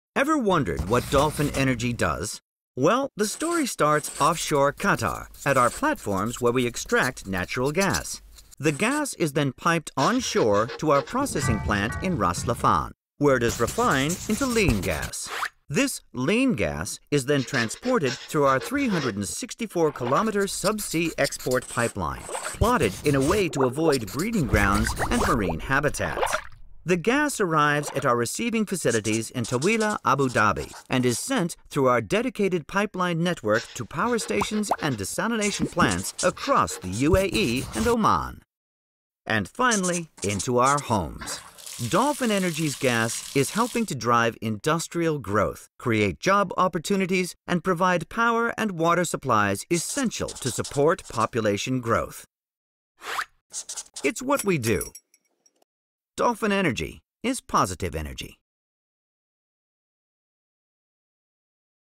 Dolphin Energy Animation Sketch TVC
Another project we would like to share, our animated sketch for Dolphin Energy's TVC.

Dolphin Energy was established by the government of Abu Dhabi to implement the Dolphin Gas Project. It involves the production and processing of natural gas from Qatar's offshore North Field and transportation of the processed gas by subsea pipeline to the UAE and Oman.